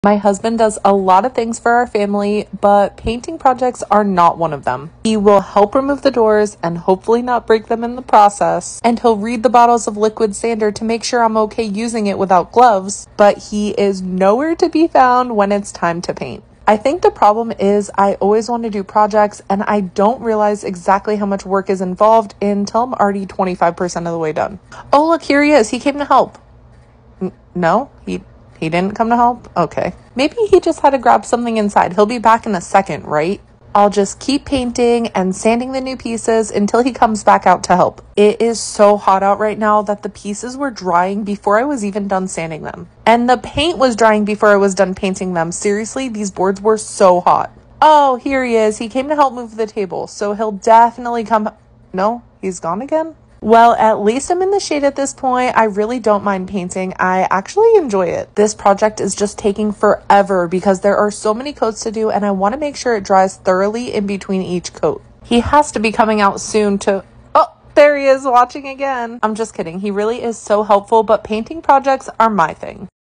Painting Our RV Cabinets: Part 1
How long do you think it’ll take to paint all the doors and cabinets in our RV because it’s a lot more than I realized 😅